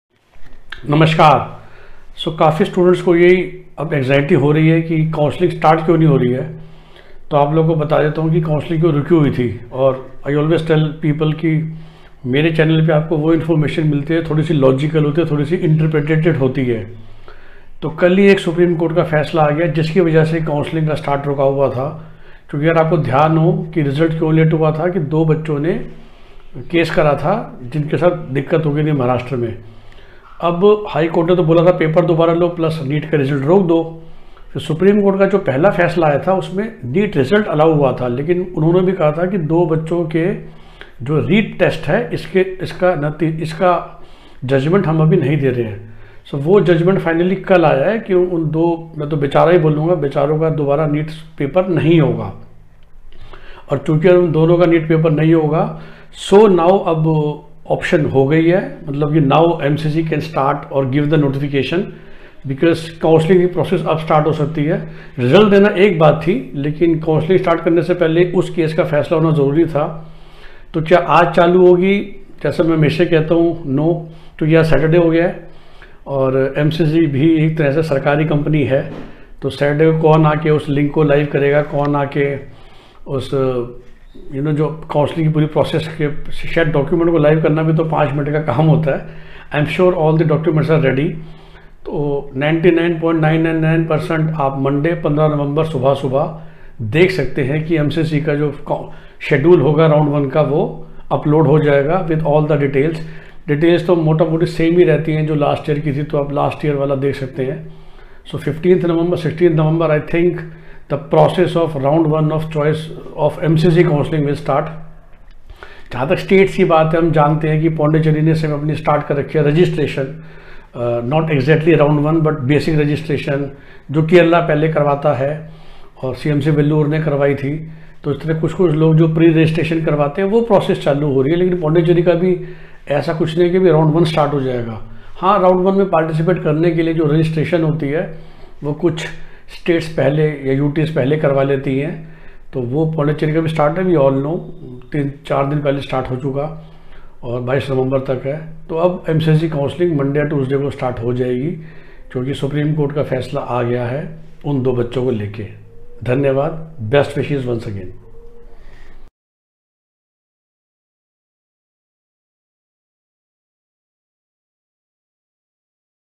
Counselling for MBBS 2021 कब शुरू होगी! SC का फैसला आ गया २ बच्चों के पेपर पर! अपनी strategy बना लें
To take our premium counselling package, call@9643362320